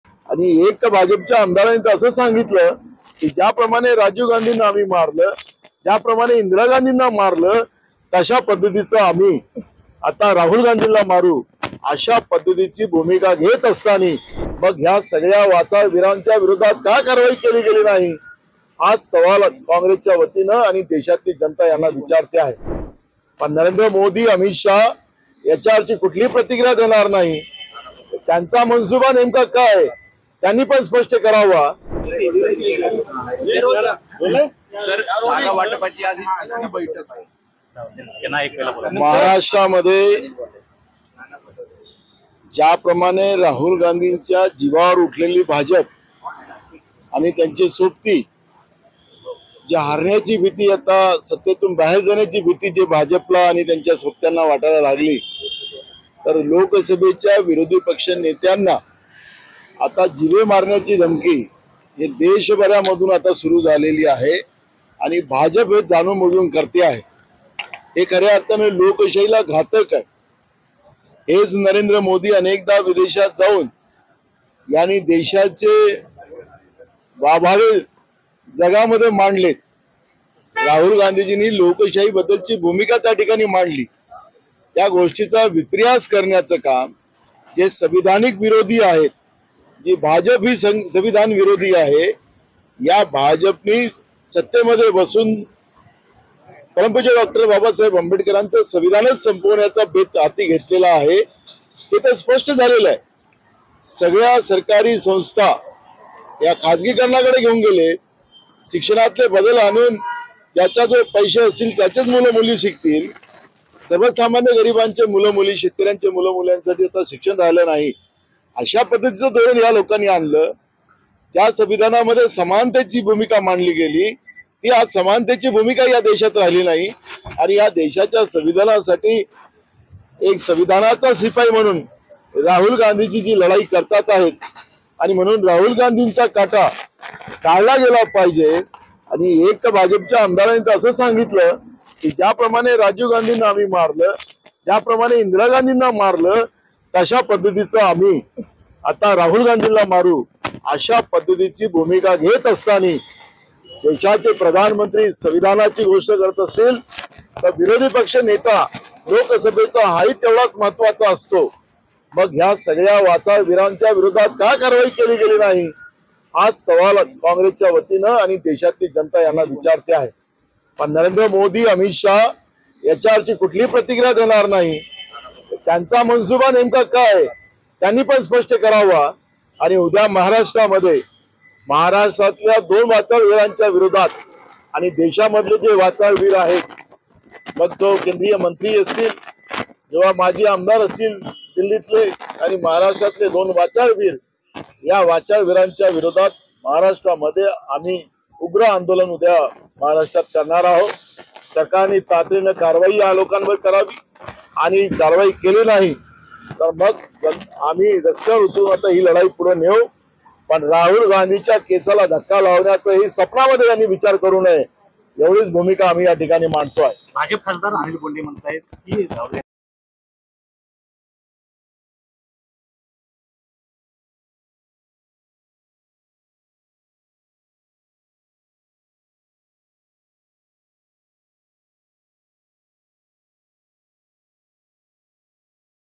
Nana Patole : राहुल गांधींविरोधात वक्तव्य, नाना पटोले भडकले.. | LetsUpp Marathi
काँग्रेस खासदार नाना पटोले यांच्याविरोधात भाजपच्या नेत्यांकडून विधान केलं जात आहेत, त्यांच्यावर कारवाई का करत नाहीत, असा खडा सवाल काँग्रेसचे प्रदेशाध्यक्ष नाना पटोले यांनी केलायं.